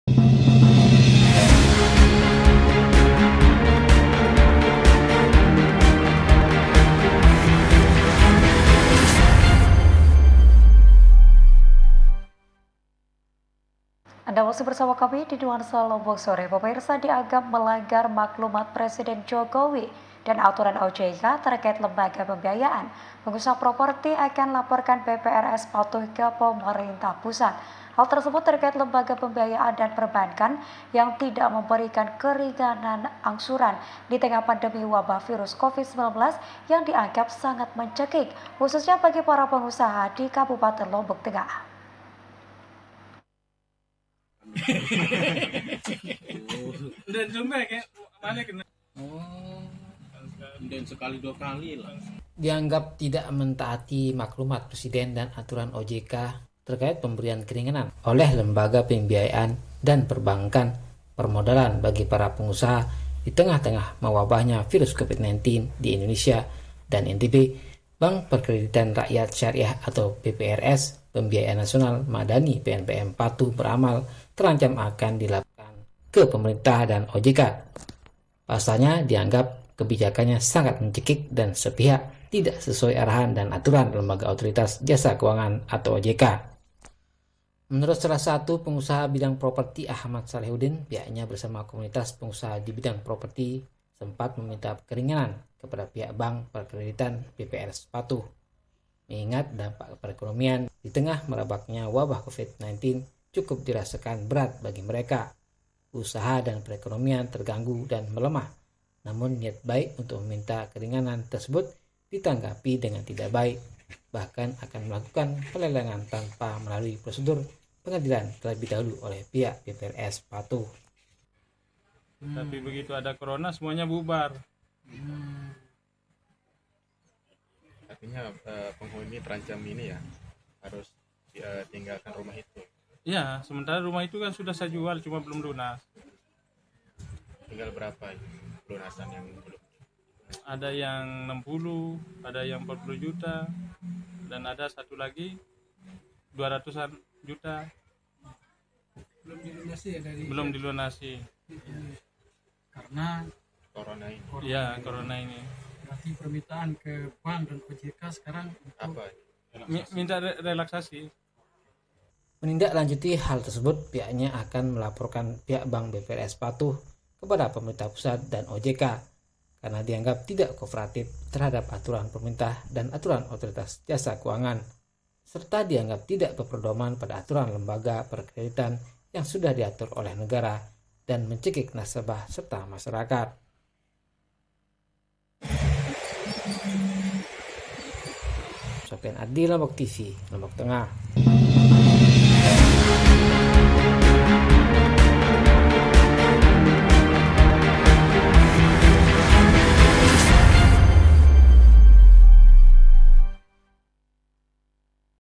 Dianggap Tidak Taat Aturan OJK,BPRS Patuh Terancam Dilaporkan
Nuansa Lombok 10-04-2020  Dianggap Tidak Taat Aturan OJK,BPRS Patuh Terancam  Dilaporkan